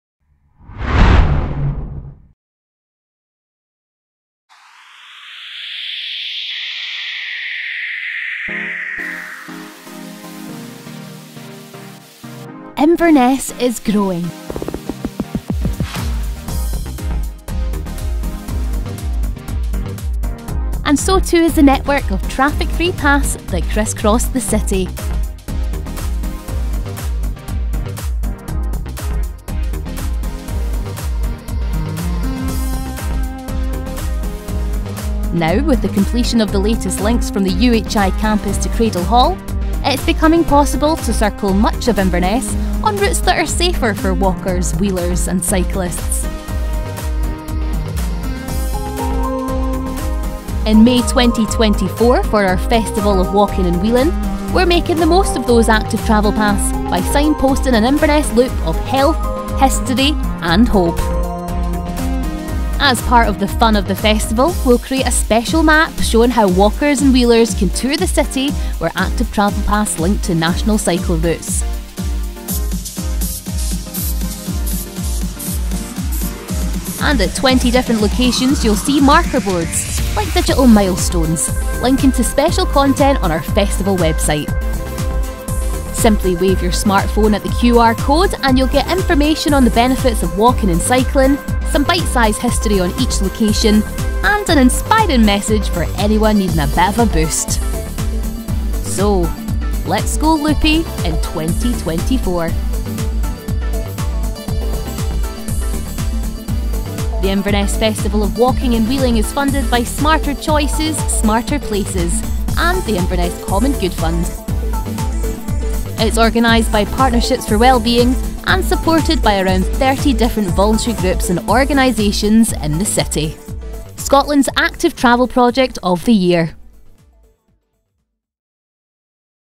The Inverness Loop of Health, History and Hope.
One of the projects we're planning for the Inverness Festival of Walking & Wheeling in 2024